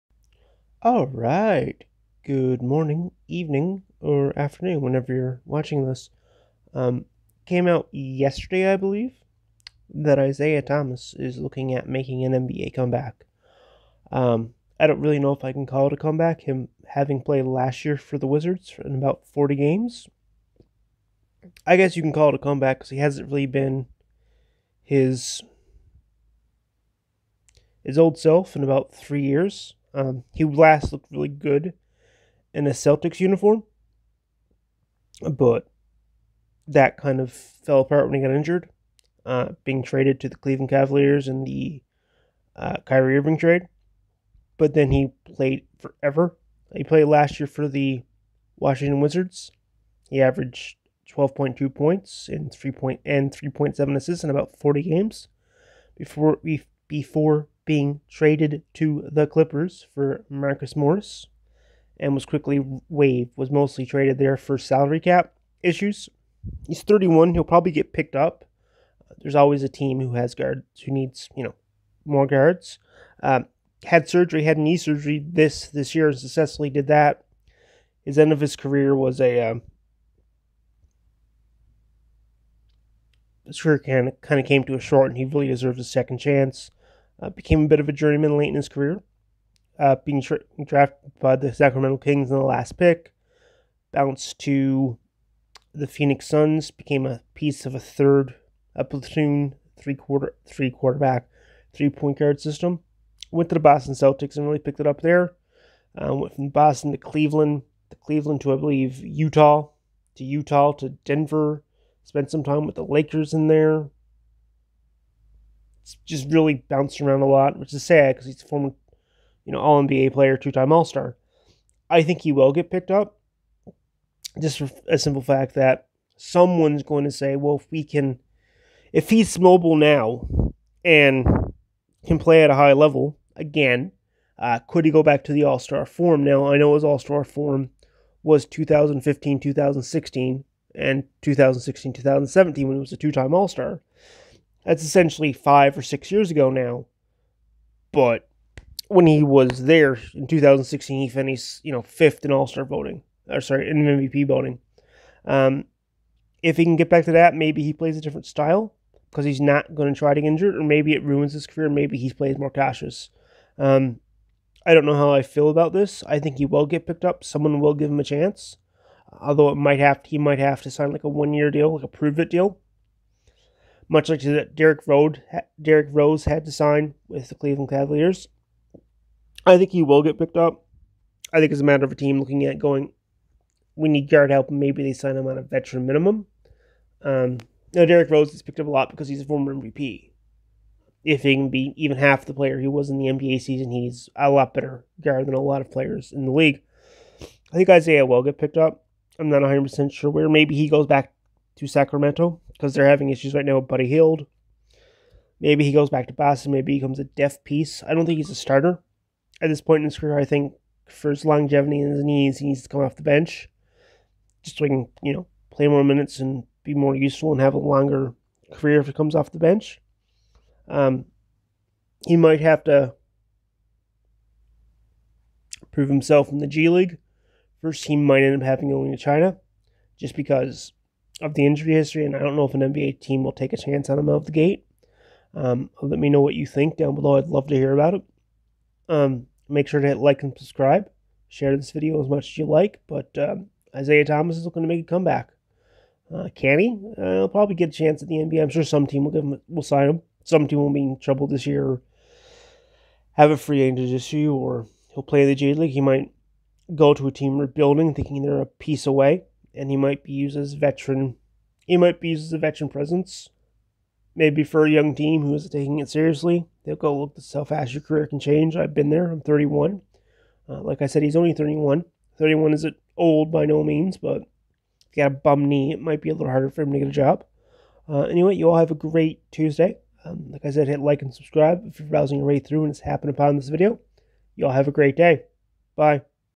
Isaiah Thomas making an nba comeback?
how hard is it to call it a comeback after last playing in the nba last year